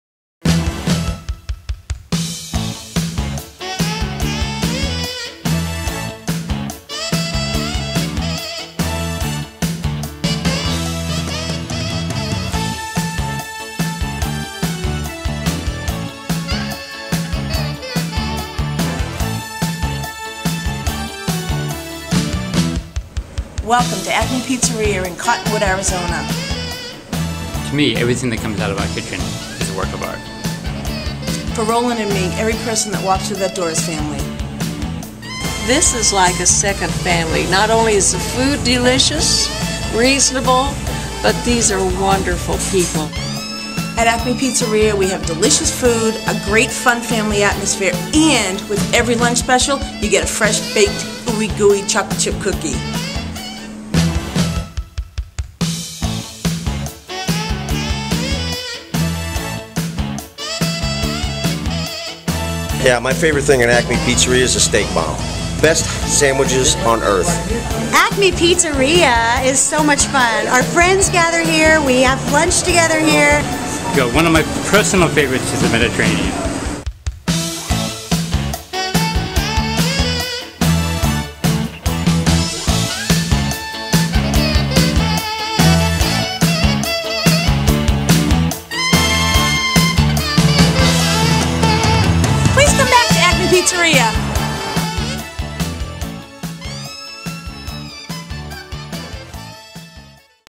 Acme Pizzaria is a fun, family restaurant with great food
Roland and Ronilee operate Acme Pizzaria in Cottonwood, Ariz. They have great food and a fun, family atmosphere. The pizzaria is located at 280 S. Main Street in Cottonwood.